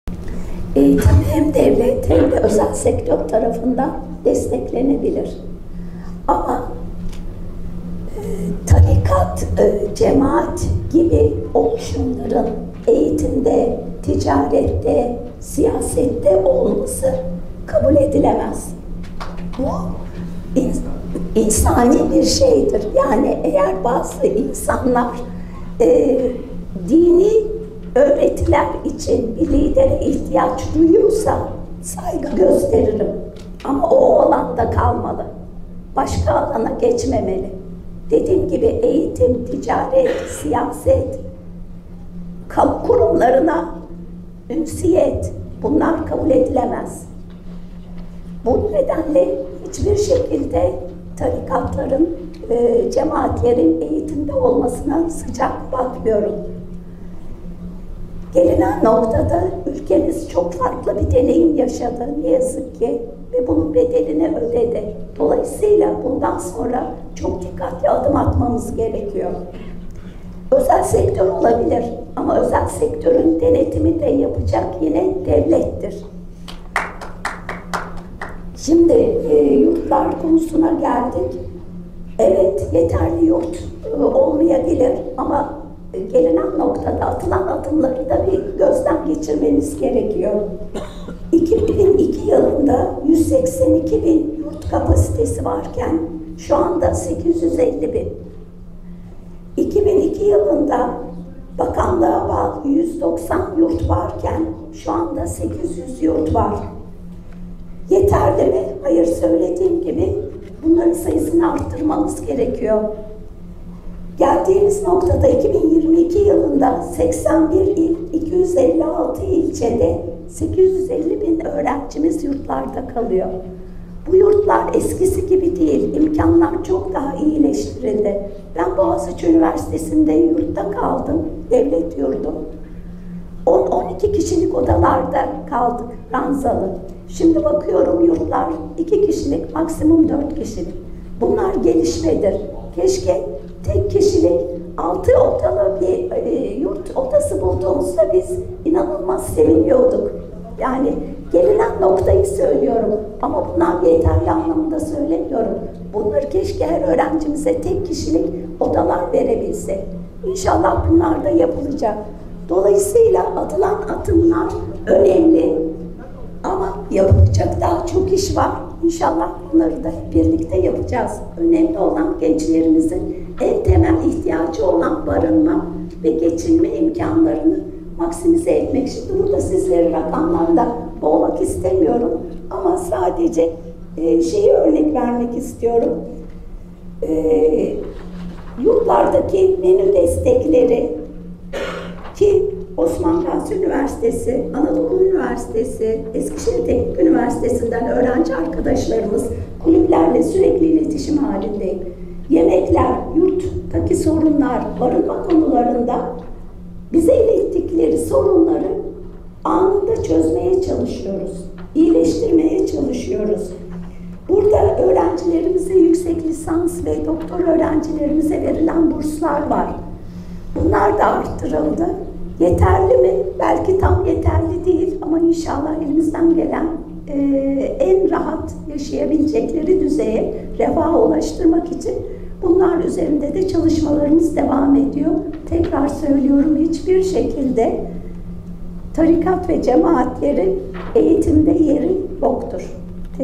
Emine Nur Günay: "Tarikat ve cemaatlerin hiçbir şekilde eğitimde yeri yoktur."
AK Parti Eskişehir Milletvekili Emine Nur Günay: "Tarikat ve cemaatlerin hiçbir şekilde eğitimde yeri yoktur."